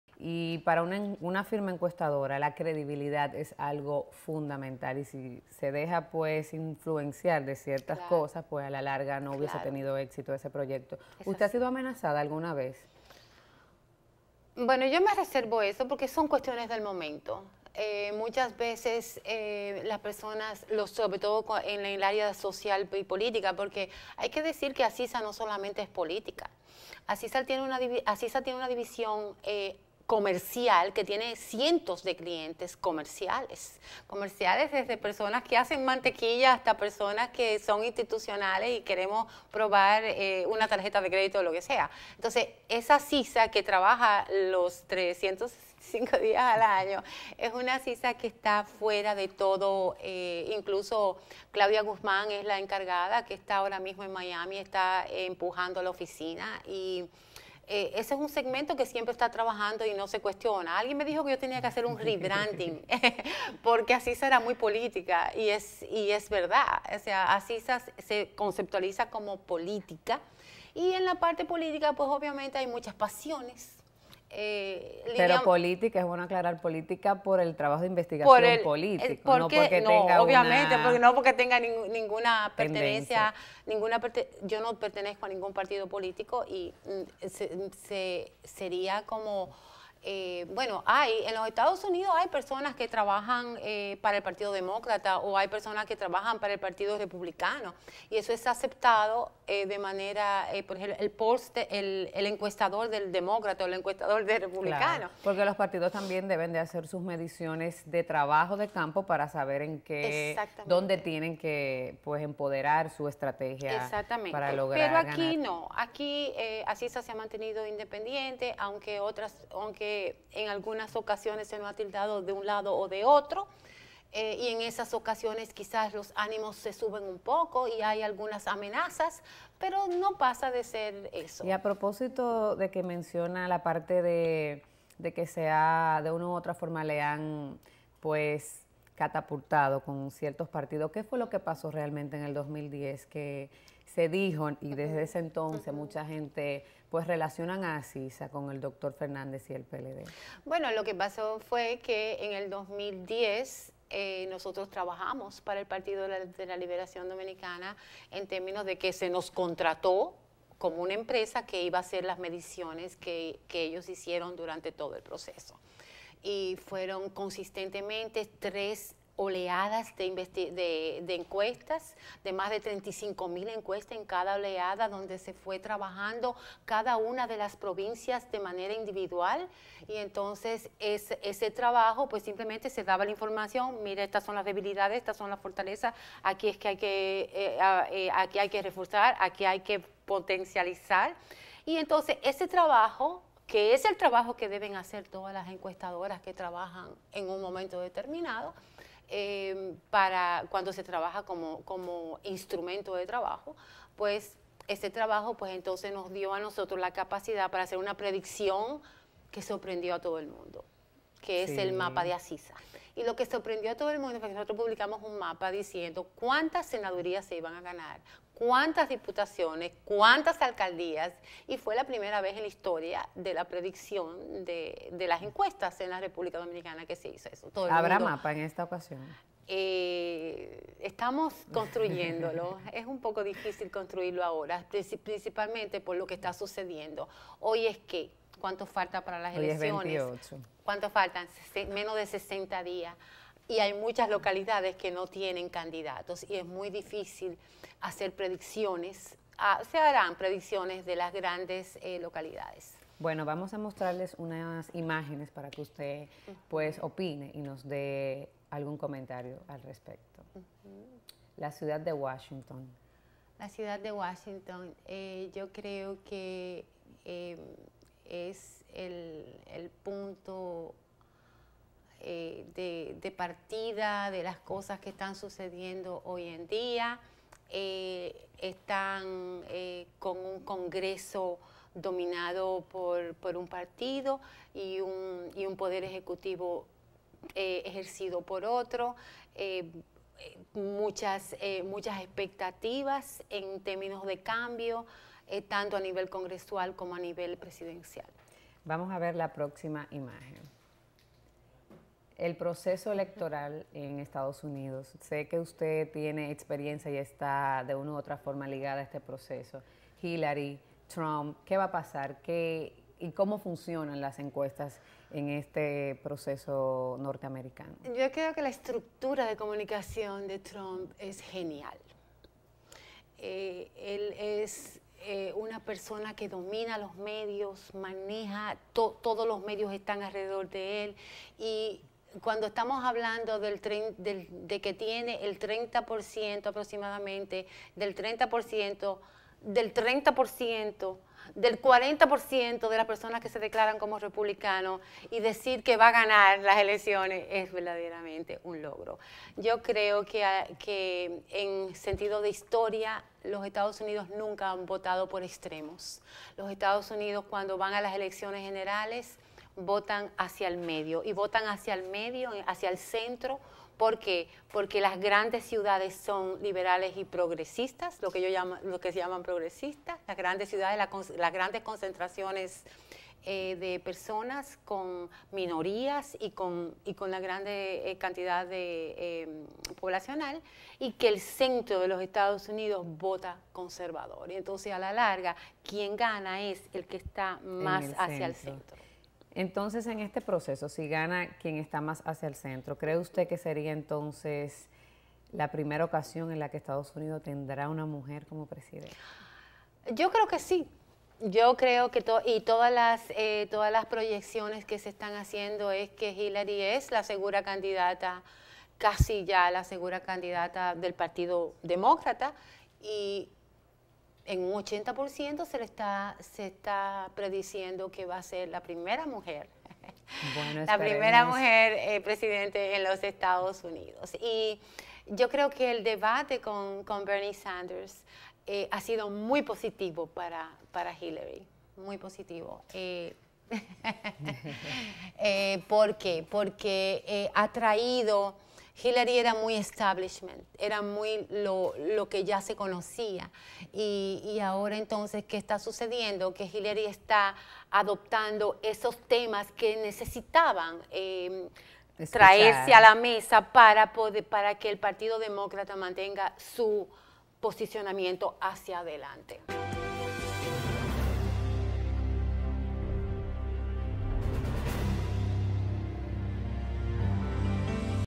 LILIAN PAGAN DIRECTORA DE ASISA Y SU PRONOSTICO DE LAS ELECCIONES 2016 (LENIS GARCIA OPINIONES)
Analizamos la importancia de las encuestas durante los procesos electorales, junto a nuestra invitada de la semana la Directora de la firma encuestadora ASISA, Lilian Pagán. Lilian Pagán es Directora de Asisa Research Group, Maestra, Académica, Psicóloga.